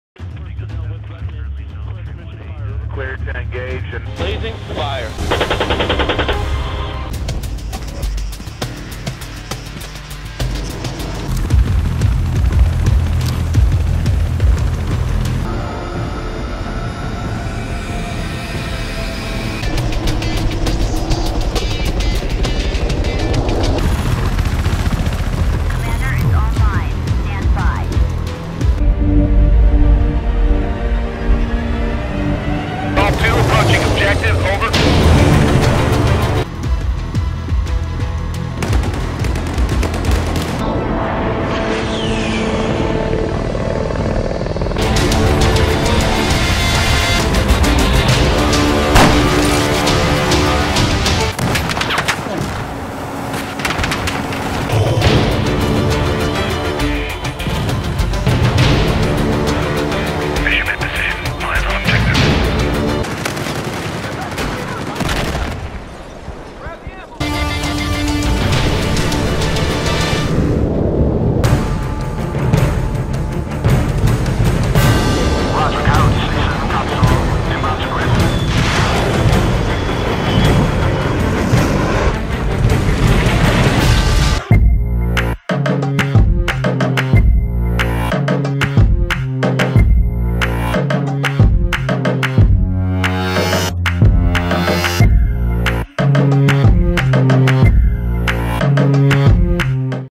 NZF Recruiting
A showcase of the games NZF.net.nz clan play. But its basically a mash up of several game trailers.